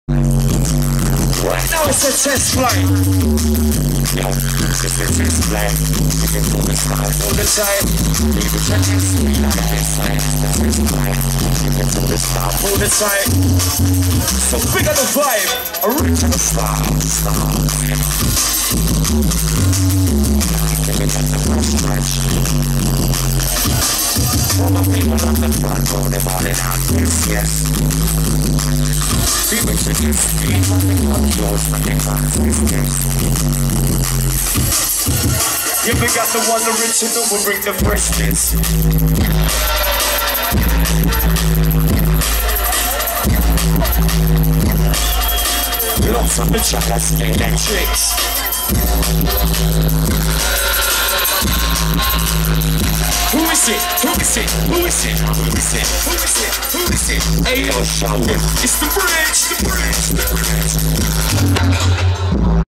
Shogun Audio OUTLOOK 2011
Shogun Audio performing @ OUTLOOK 2011 on the dock side stage